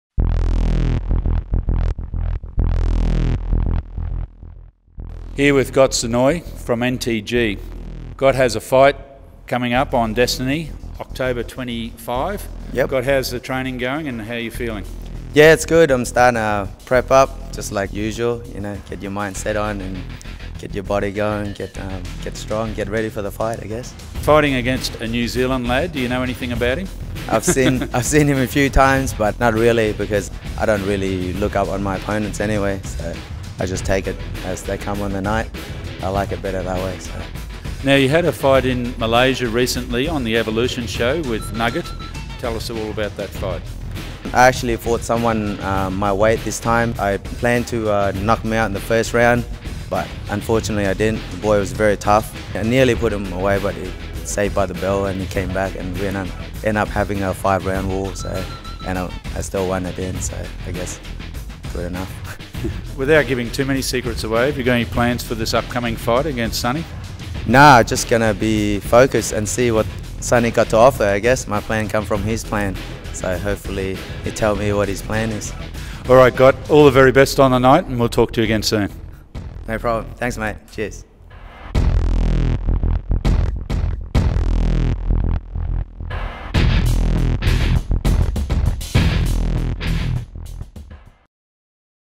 Ghot Seur Noi Fights Sone Vannathy on the Destiny 3 Fight Card
UppercutTV Talks to Ghot Seur Noi of NTG before his fight on Destiy 3 - 25 October.
Also keep up to date with all the latest MMA, Muay Thai and Combat Sports News.